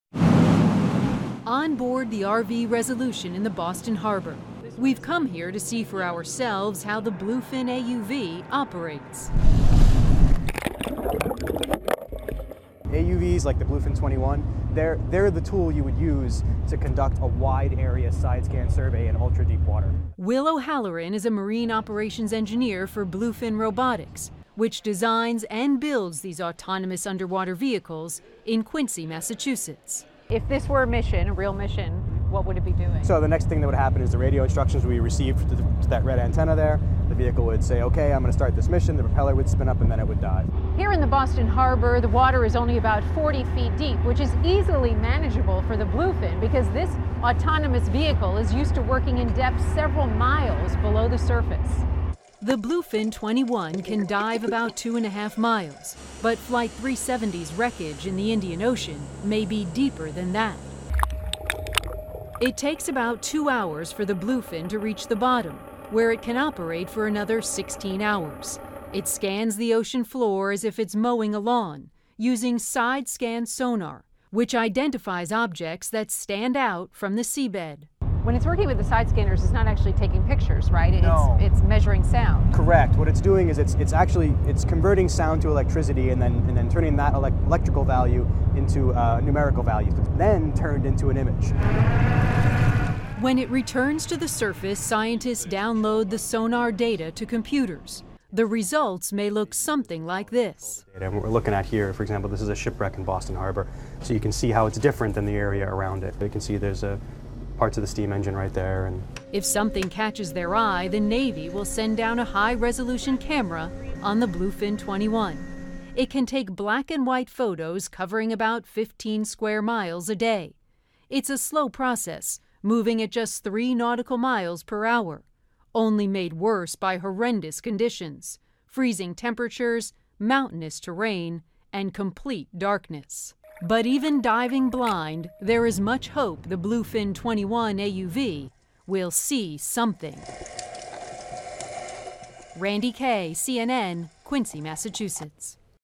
See how the Bluefin-21 AUV works underwater
CNN's Randi Kaye shows how the Bluefin-21 AUV works.